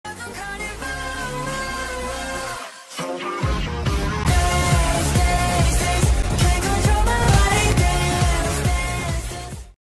𝙲𝚊𝚗’𝚝 𝚌𝚘𝚗𝚝𝚛𝚘𝚕 𝚖𝚢 𝚋𝚘𝚍𝚢 | 𝙳𝚛𝚊𝚛𝚛𝚢 | 𝙰𝚖𝚘𝚛𝚝𝚎𝚗𝚝𝚒𝚊 𝙳𝚛𝚊𝚌𝚘
Why is draco under amortentia? 🧐

becoz

b because

yes

(snape wont do anything because he cant harm draco as promised)

edit: 9:33 PM PST

HELP HSJHWJS I FORGOT HARRY’S GLASSES ? UHM LETS JUST PRETEND IT FELL FROM THE ROOF